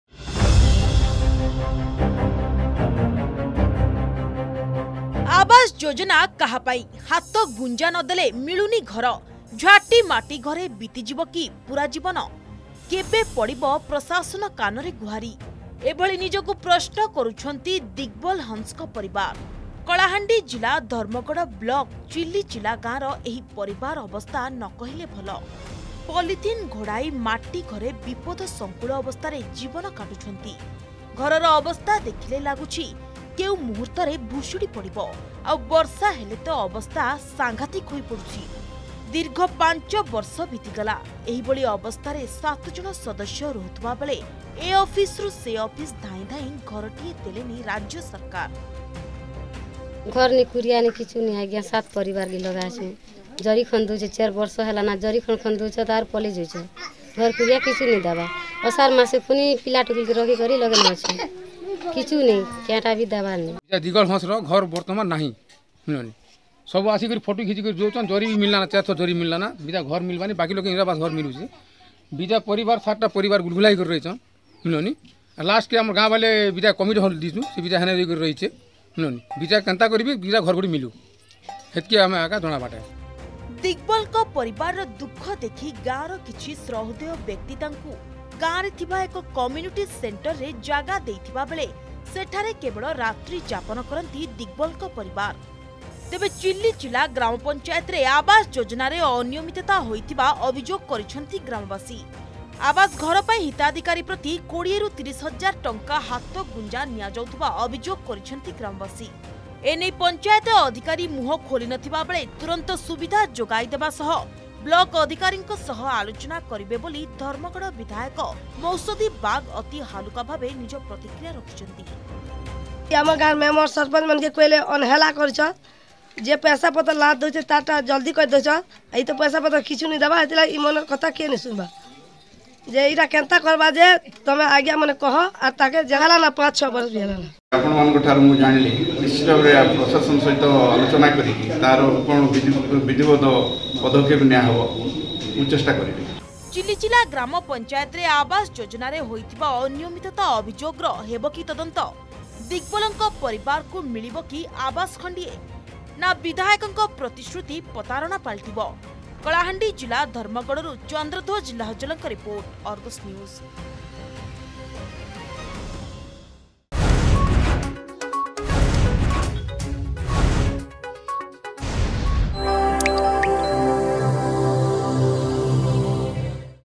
ଆବାସ ଯୋଜନା କାହା ପାଇଁ, ଝାଟିମାଟି ଘରେ ବିତିଯିବ କି ପୁରା ଜୀବନ?
ଝାଟିମାଟି ଘରେ ବିତିଯିବ କି ପୁରା ଜୀବନ?
କେବେ ପଡିବ ପ୍ରଶାସନ କାନରେ ଗୁହାରି?
ଏଭଳି ନିଜକୁ ପ୍ରଶ୍ନ କରୁଛନ୍ତି ଦିଗବଲ୍ ହଂସଙ୍କ ପରିବାର । କଳାହାଣ୍ଡି ଜିଲ୍ଲା ଧର୍ମଗଡ ବ୍ଲକ ଚିଲିଚିଲା ଗାଁର ଏହି ପରିବାର ଅବସ୍ଥା ନକହିଲେ ଭଲ । ପିଲିଥିନ୍ ଘୋଡାଇ ମାଟି ଘରେ ବିପଦ ସଙ୍କୁଳ ଅବସ୍ଥାରେ ଜୀବନ କାଟୁଛନ୍ତି ।